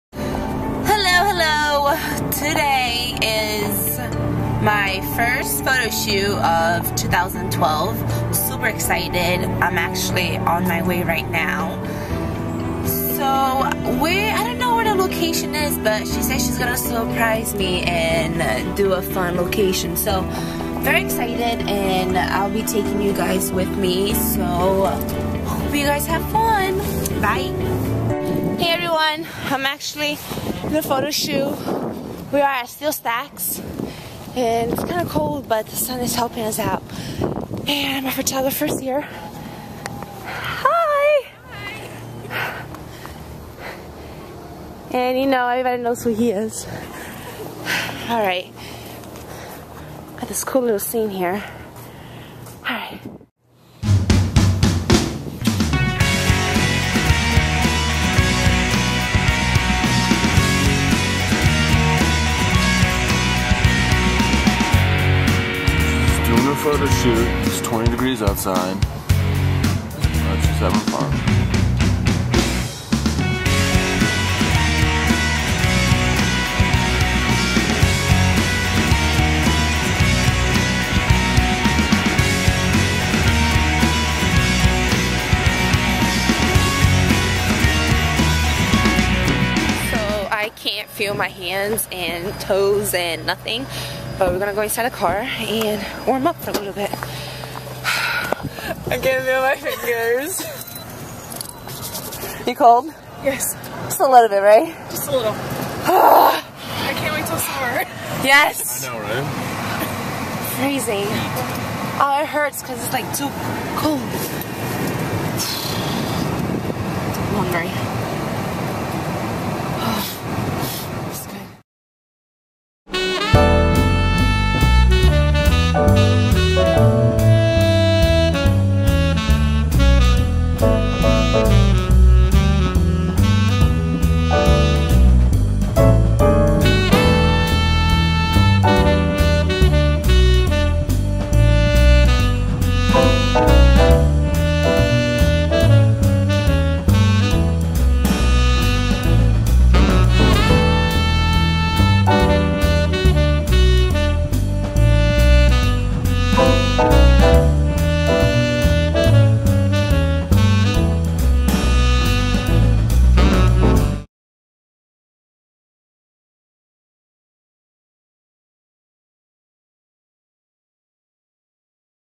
Photoshoot super cold
Jan 20,2012 I went for my first photoshoot of 2012.. super excited!!! Thank you Sonya Christman!